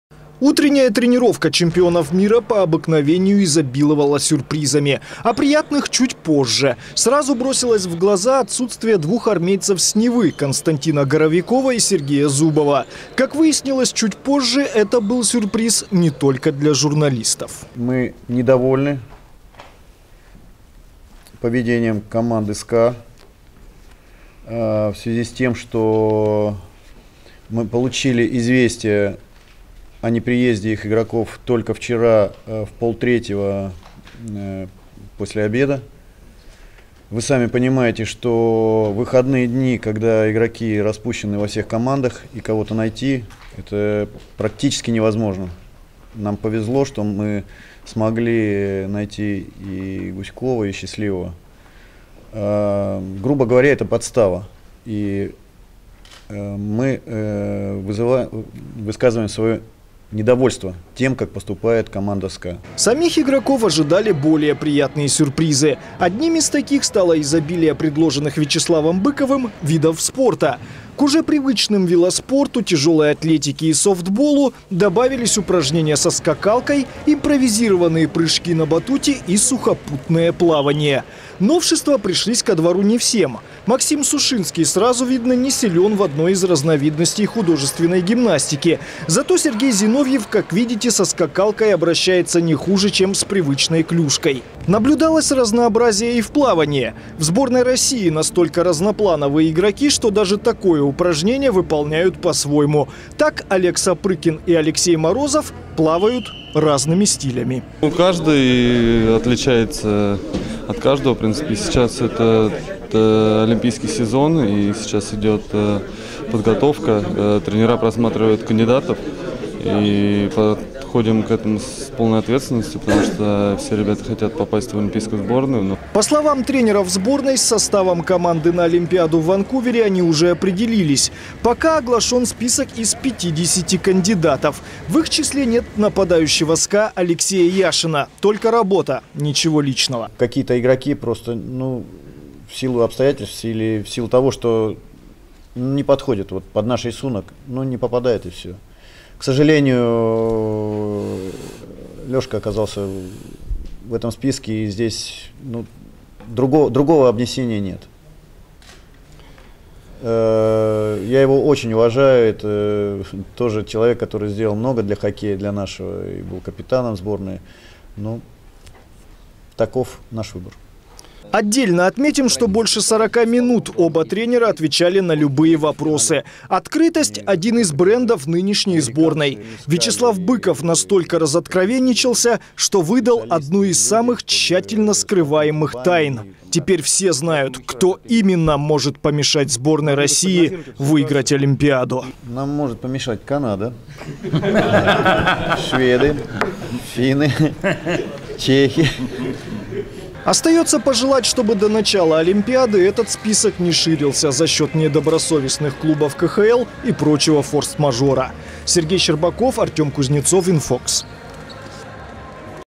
СКА подставил сборную России
«Подставой» назвал главный тренер сборной России Вячеслав Быков решение руководства хоккейного клуба СКА не отпускать игроков команды Сергея Зубова и Константина Горовикова в расположение национальной команды, которая на базе в Новогорске готовится к Кубку «Карьялы». Больше возмущение вызвал даже не сам факт отказа в приезде игроков, а то обстоятельство, что оповещен тренерский штаб сборной был лишь за день до начала сборов.
Дата публикации: 3 ноября 2009 19:59